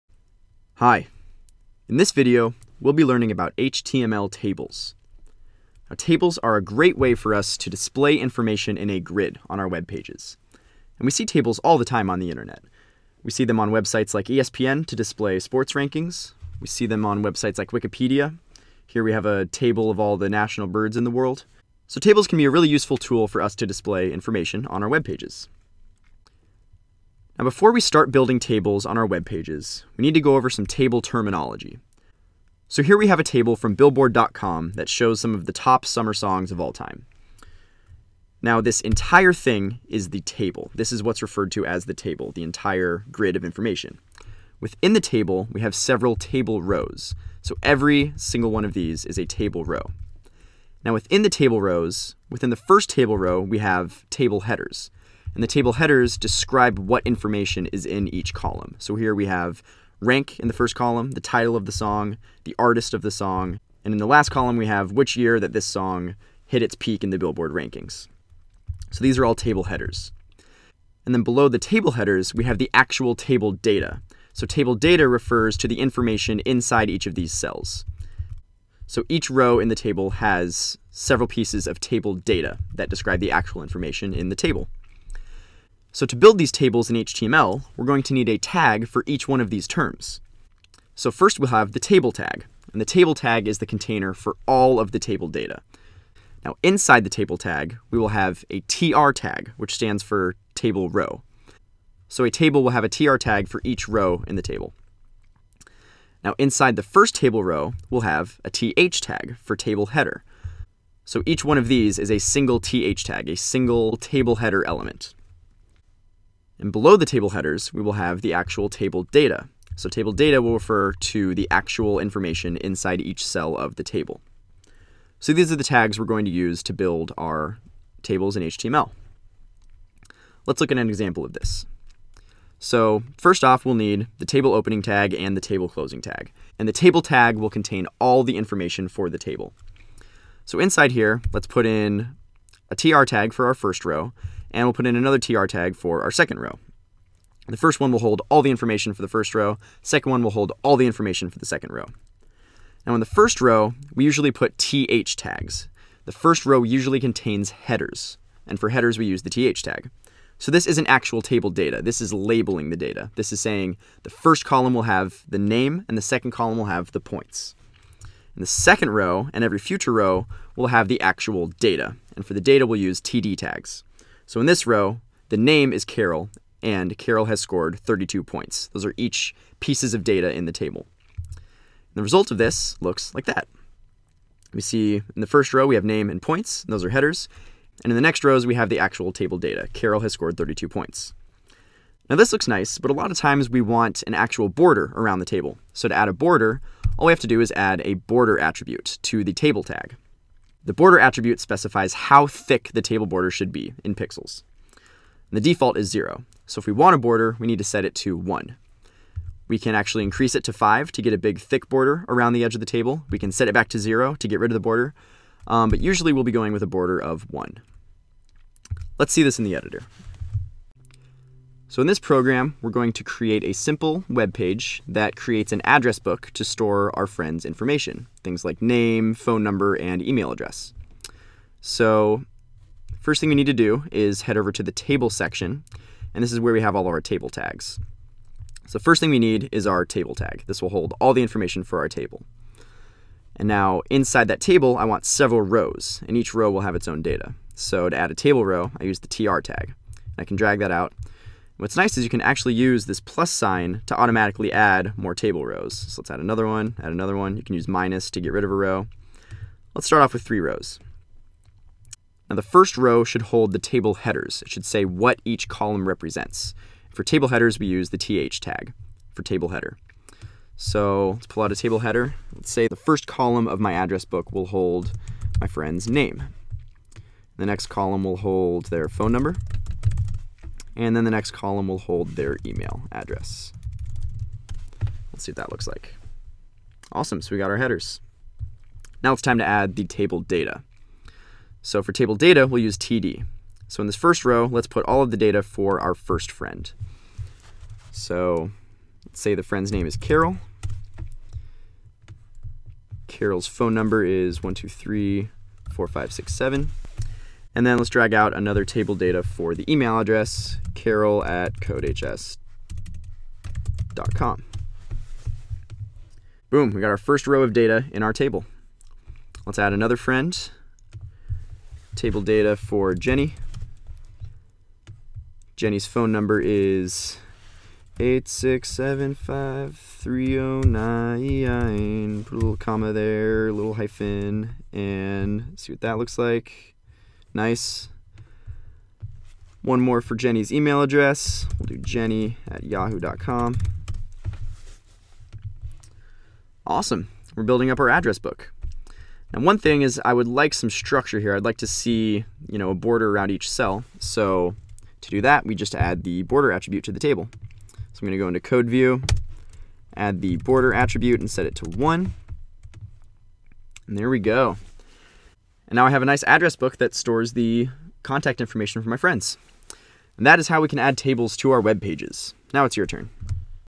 HTML Tables
In this video, we learn how to add tables to our web pages.